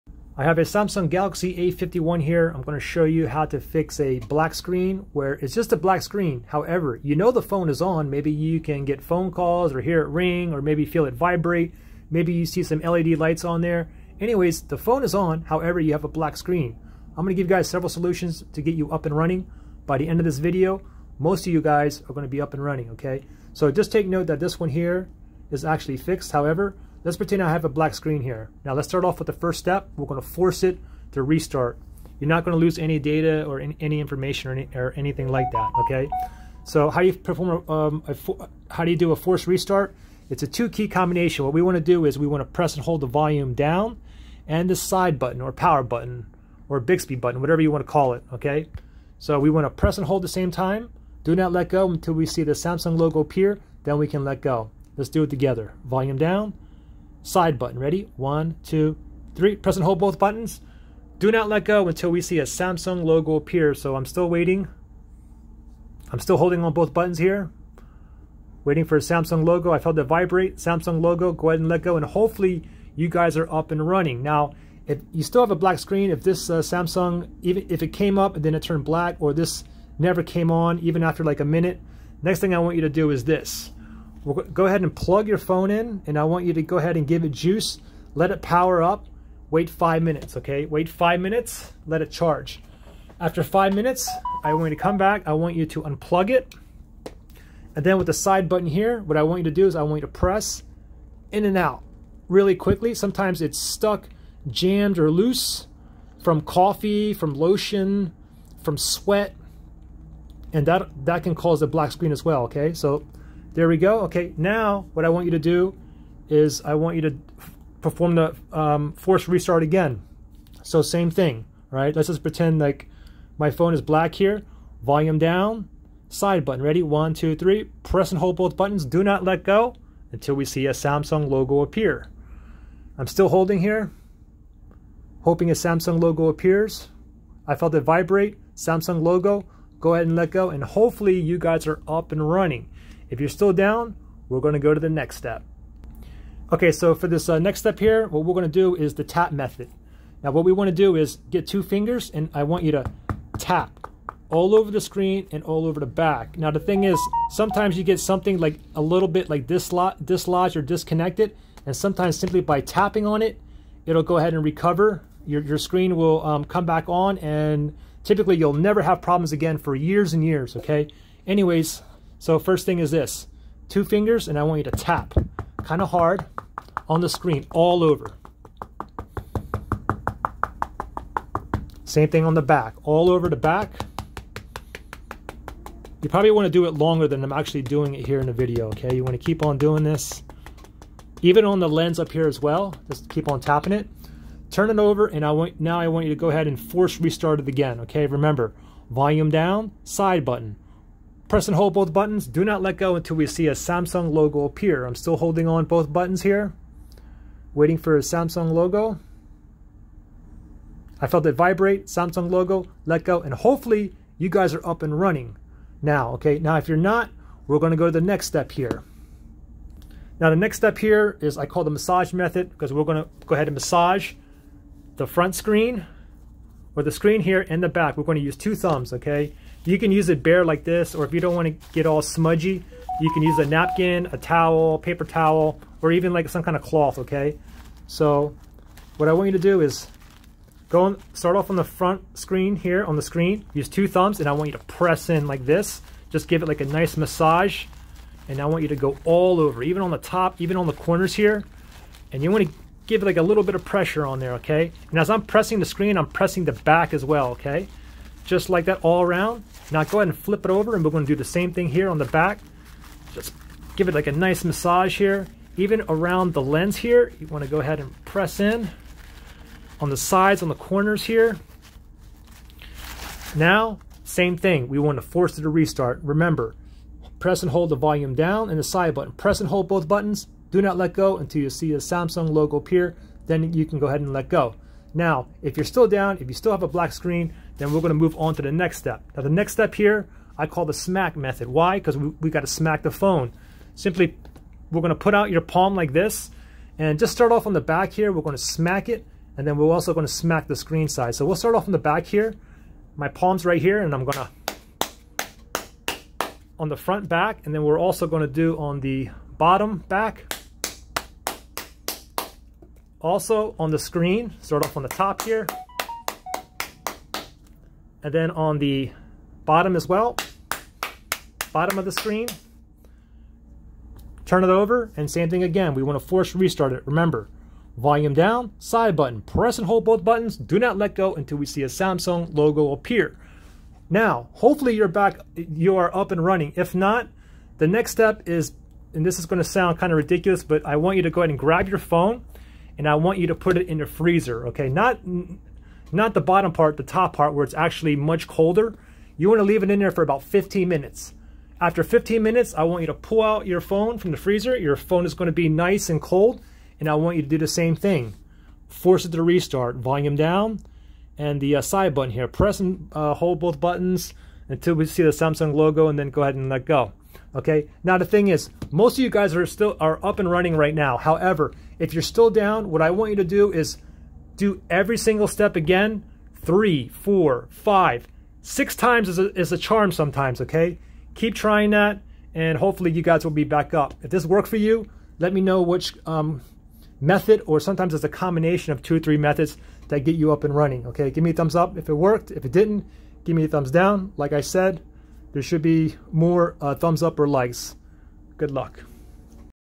Galaxy A51: How to Fix Black Screen or Screen Won't Turn On (6 Easy Fixes)
Note: Do NOT press the power button more then 4 times in a row or it will call emergency services if you have that enabled.

I give you 6 easy fixes on how to fix a black screen or screen won't turn on on a Samsung Galaxy A51. Note that you have a black screen but you know the phone is on as you can maybe feel it vibrate, see LED lights, or can hear it ring. Make sure to follow each method and by the end of the video most of  you will have a working screen. If these methods did not work for you then try every step 4, 5, even 6 more times. Good Luck!

Samsung Galaxy A51 Factory Unlocked Cell Phone | 128GB of Storage | Long Lasting Battery | Single SIM | GSM or CDMA Compatible | US Version